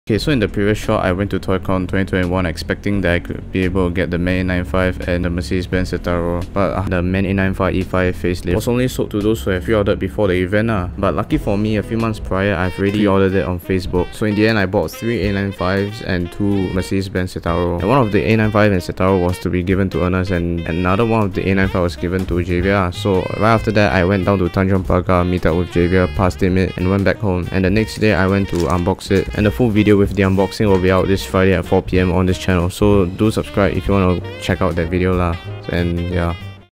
Buying the Tiny Buses (SG ToyCon 2021 Part 2)
In this short, Jin manages to buy what he needed from the event.

Oof this is a auto generated descriptions from the depths of youtube.